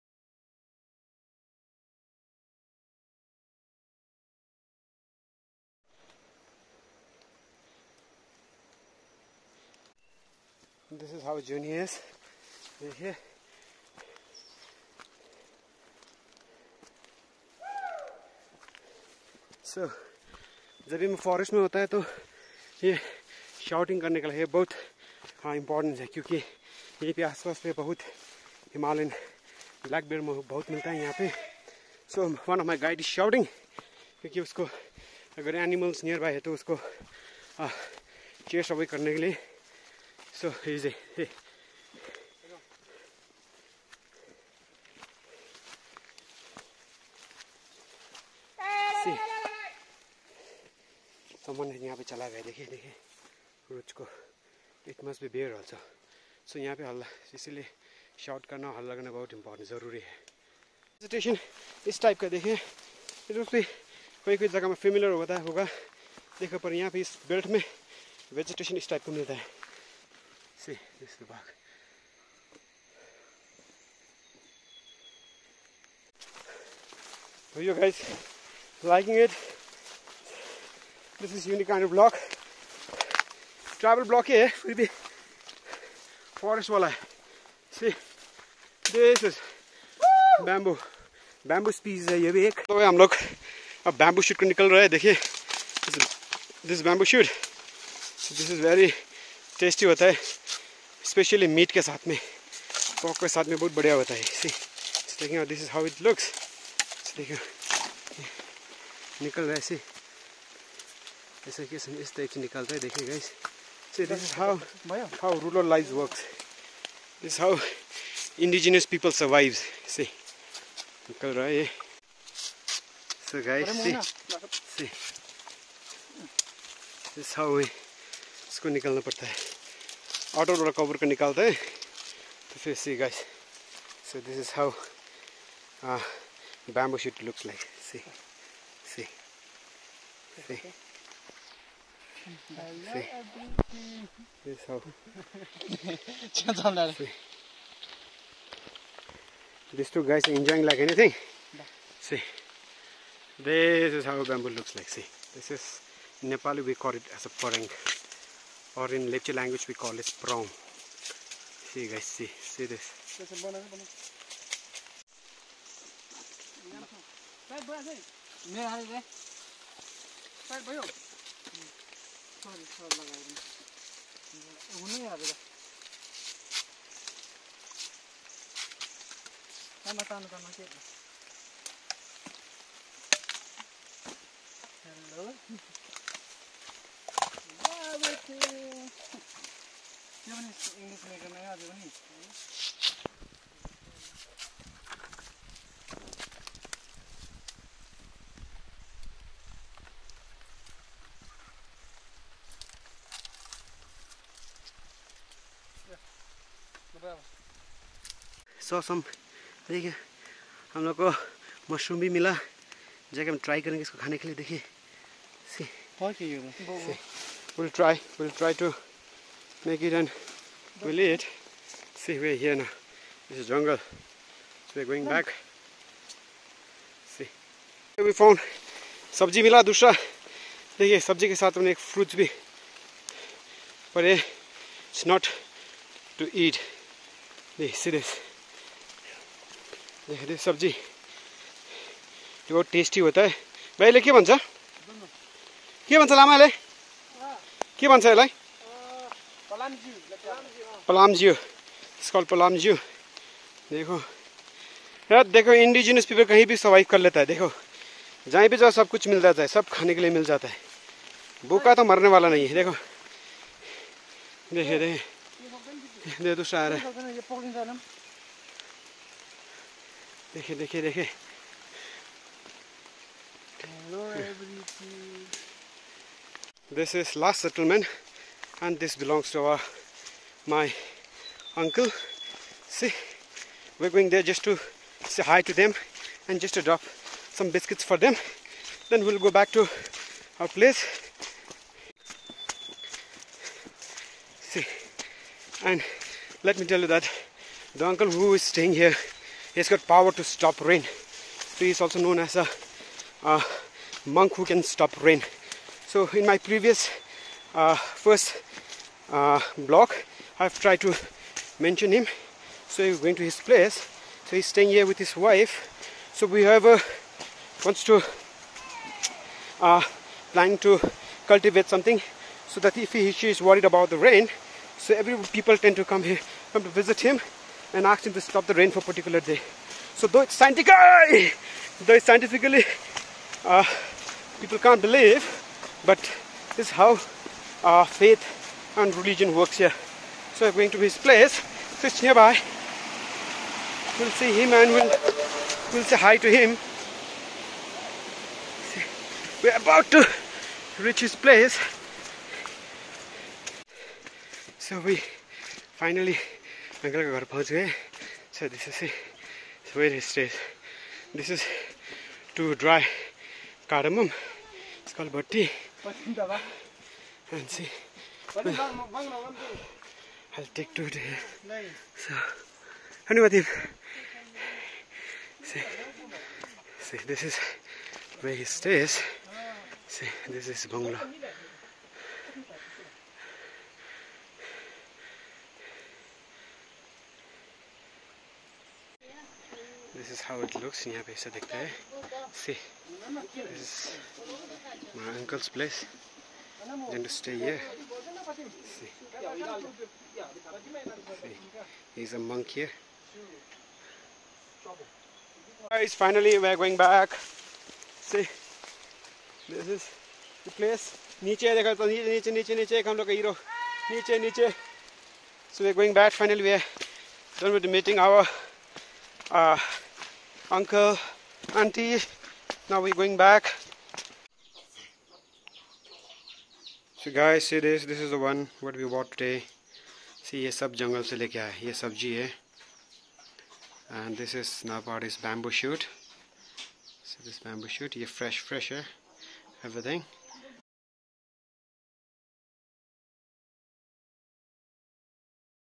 collecting veggies from the woods
Since time immemorial Indigenous people have been depending on mother nature for their livelihood. Even in this era of globalisation indigenous people still depends on her for their survival.
This vlog is about collecting of veggies from the woods in the one of the jungle of Lingthem, Upper Dzongu North Sikkim (India). The very jungle in the vlog is famous for the veggies and wild animals like Himalayan black bears. The jungle is abundance with different types of  veggies and wild fruits like nettles, furnes, shoots, bamboo shoots, roots, wild avocado, wild nuts along with different types of medicinal plants.